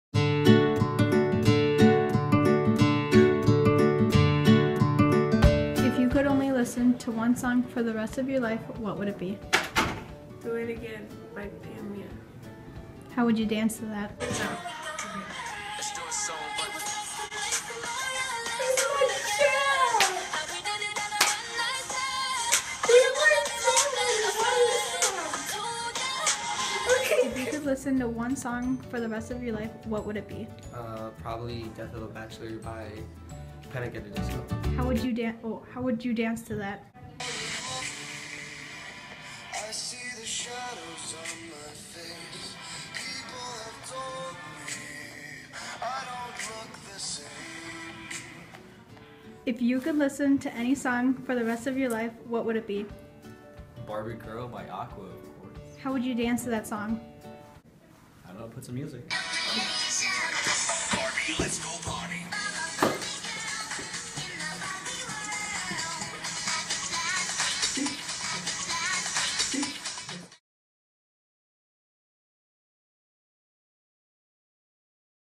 VeritazzFeed: Dance Music
Watch Karina, Daniel, and Pedro dance to their favorite jams.  It's a total LOL moment!  Produced by the Veritas HIgh School Journalism class.